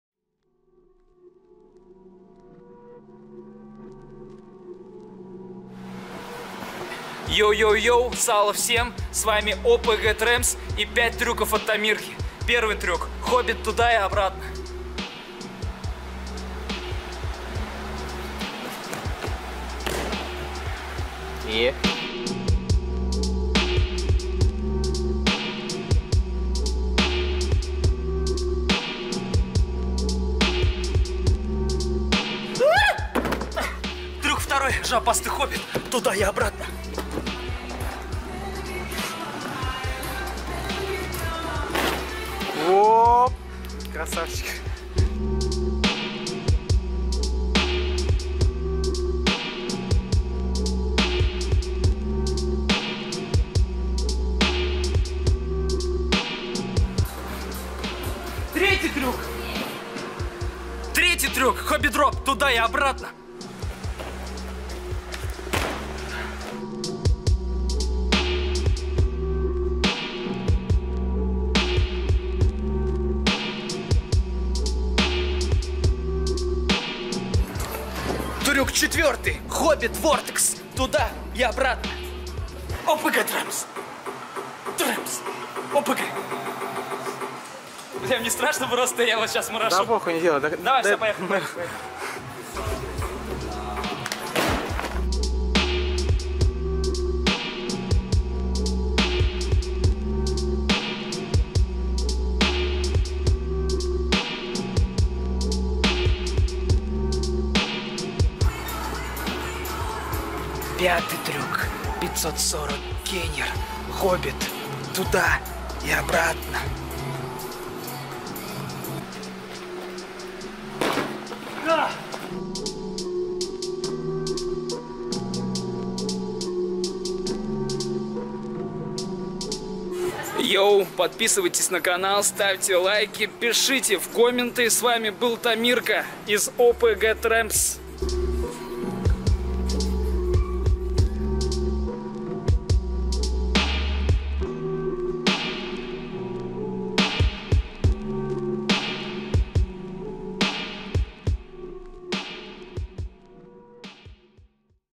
5 TRICKS - TAMIR | TRAMPS SHOW ( Episode #4)
Новое шоу ОПГ ТРЭМПС.
В выпусках этого шоу, мы будем стараться придумывать абсолютно новые трюки. Показывать вам, и тем самым, развивать свои навыки фантазии в паркуре.

Выпуск с Тамиром и его 5 трюками. Надеемся, что он придется вам по душе. Сап!
New show TRAMPS GANG.
In the releases of this show, we will try to invent brand new tricks. Show you, and thereby develop your fantasy skills in parkour.

Еdition with Tamir and his 5 tricks. We hope that you will like it.
SUP!